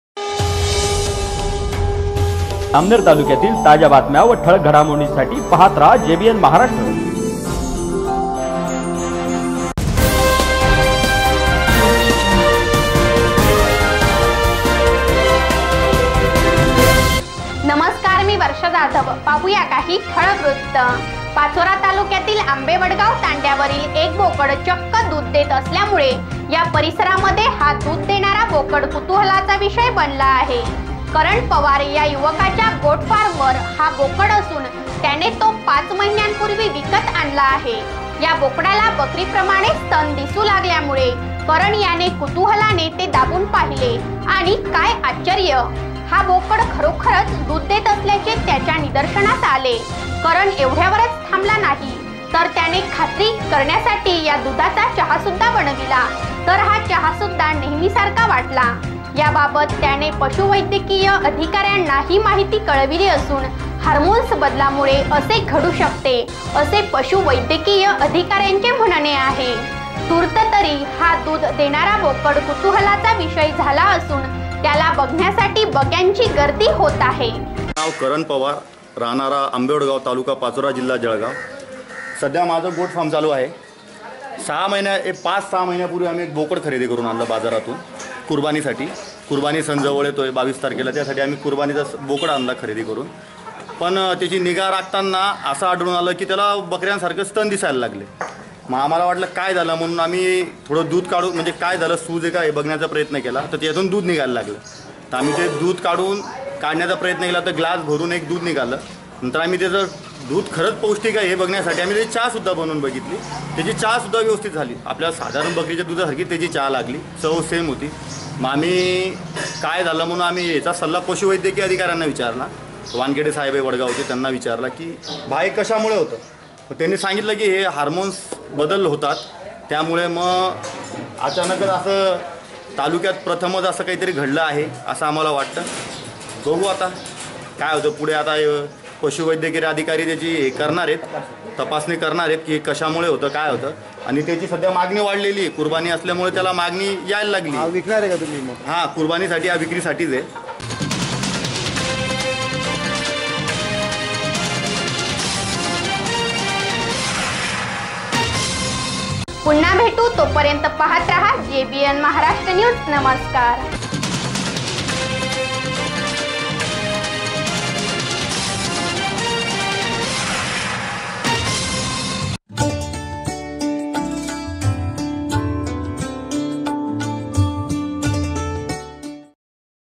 बोकड देतोय दुध jbn maharashtra news jalgaon
आश्चर्यम् ! ... चक्क बोकड देतोय दुध